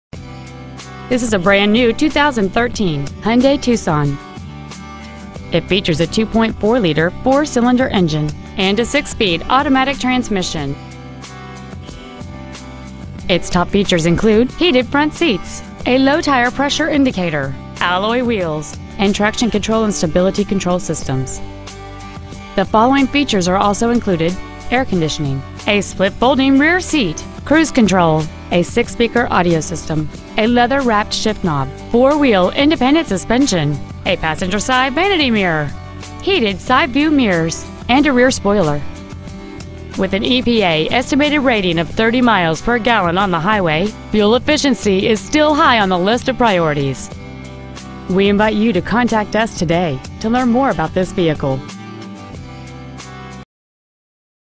2013 Hyundai Tucson Miami FL
We are proud to present this 2013 Hyundai Tucson .

 Year : 2013
 Make : Hyundai
 Model : Tucson
 Engine : 2.4l i4 mpi dohc cvvt 16v theta 2
 Trans . : 6-speed automatic with overdrive
 Exterior : chai bronze
 Miles : 10
 Stock : T678193
 Interior : chai bronze

 Lehman Mazda